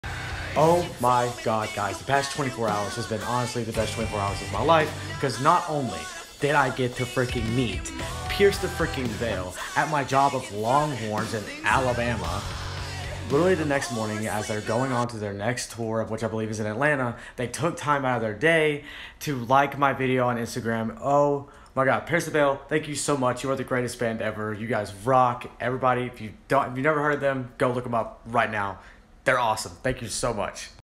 Pierce The Veil liked my picture!!!
Pierce The Veil liked my picture!!! Not only did I get a picture with Pierce The Veil, they also liked my picture!!! I’m so excited